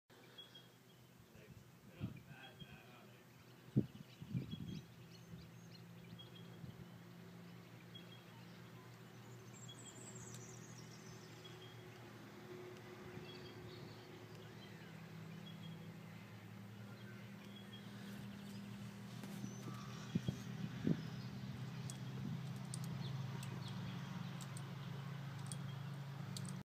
Our Very Sleepy Horse!
Our horse Roxy takes a nap standing up. Usually not a problem for horses ... except she nearly falls over! :P
This is actually a common occurrence here at Chapman Valley Horse Riding with Roxy. It's her quirk!